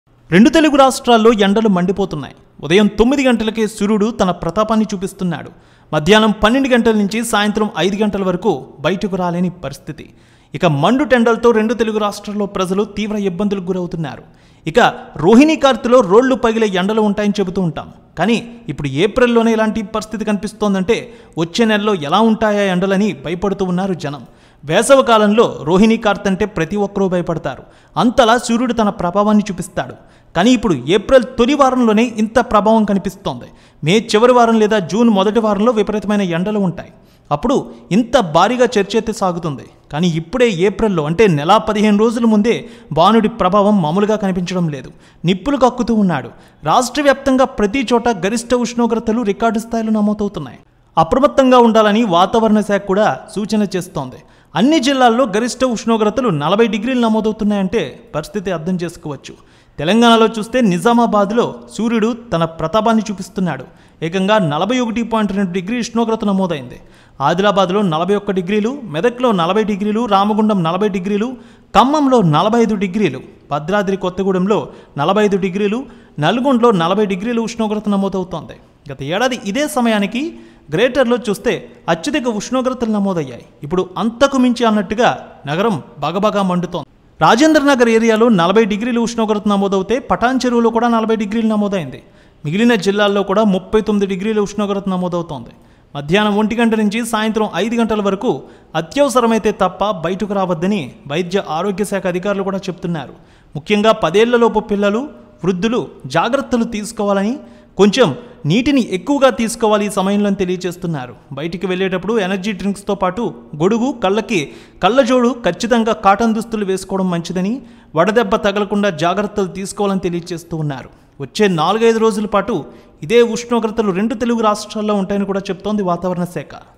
IMD Hyderabad Issues Heat Wave Warning As Temperatures May Reach 44 Degrees Celsius|Naya News
Watch IMD Hyderabad Issues Heat Wave Warning As Temperatures May Reach 44 Degrees Celsius|Naya News